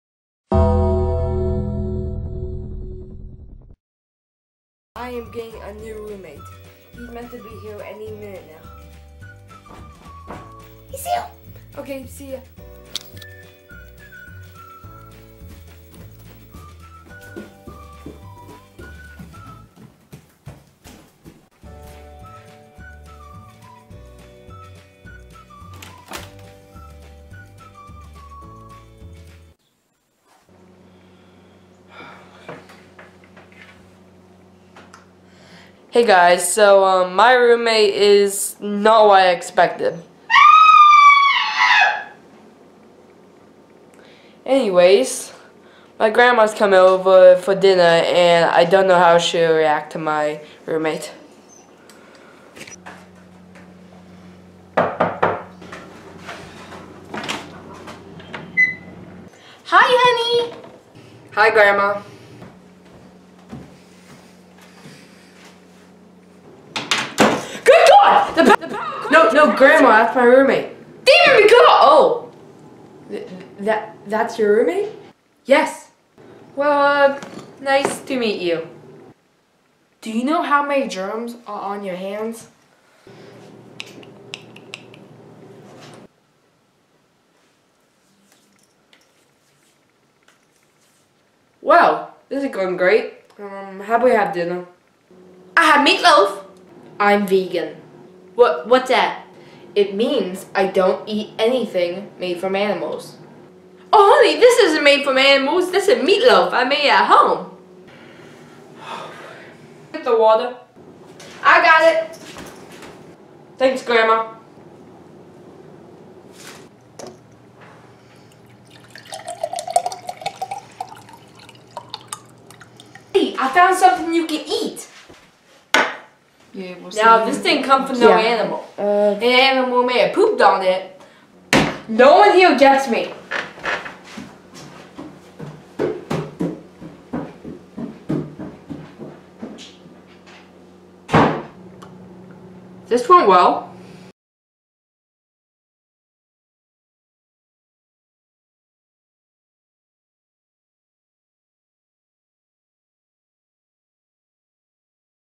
MY NEW ROOMMATE! ep 2 | Grandma meets Eddie
In this video Eddie meets Paytons grandma for dinner.